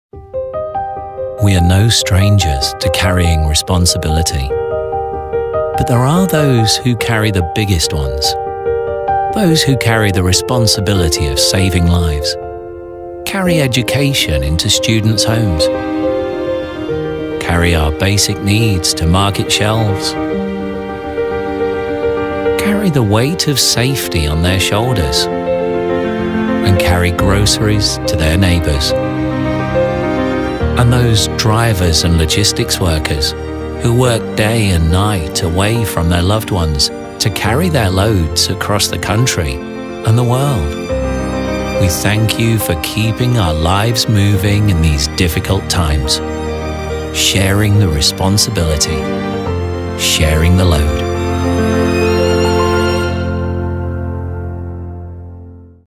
VO: Guy Harris / FORD Trucks 'A Million Thanks To...'
Reklamveren: FORD Trucks
Reklam Ajansı: Happy People
Seslendirmen: Guy Harris